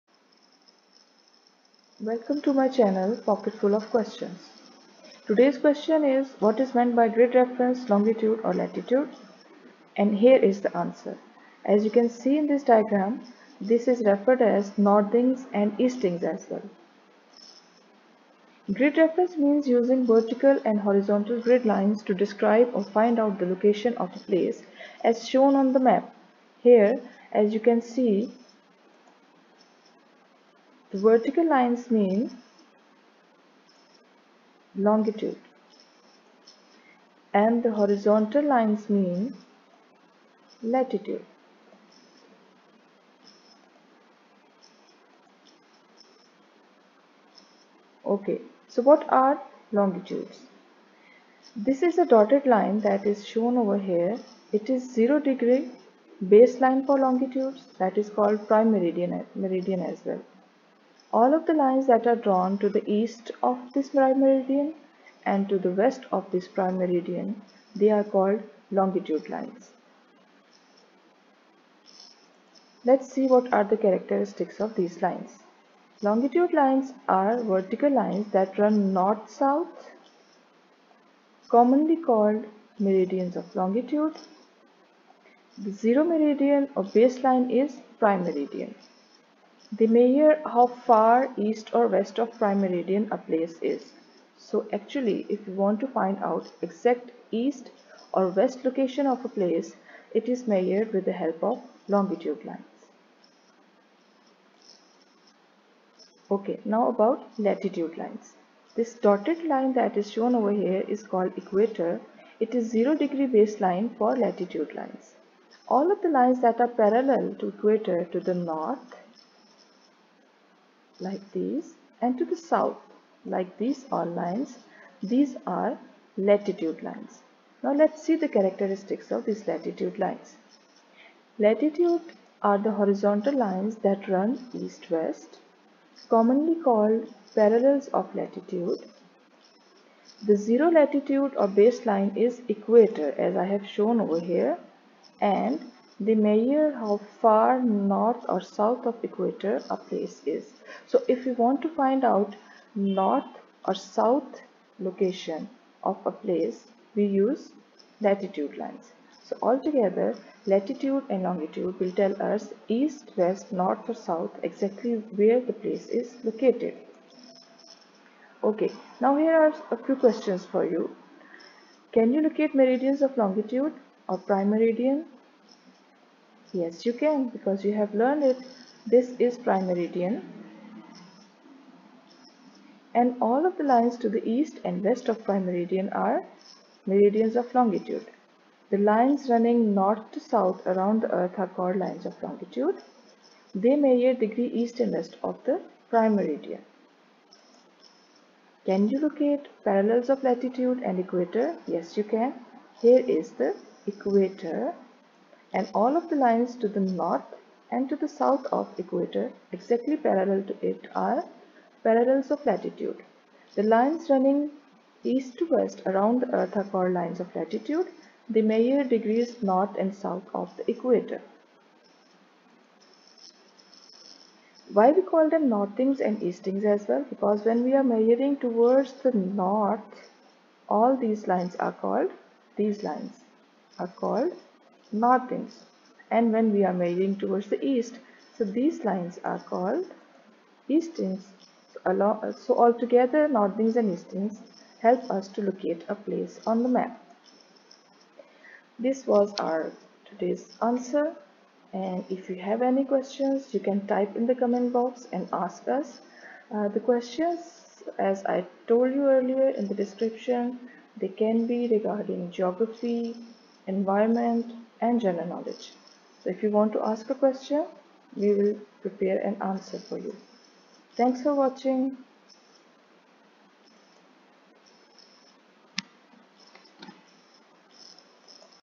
What is Grid reference, longitude, and latitude?
Let's learn how to find the location of a place on a map by using Grid reference, Meridians of Longitude, and Parallels of Latitude.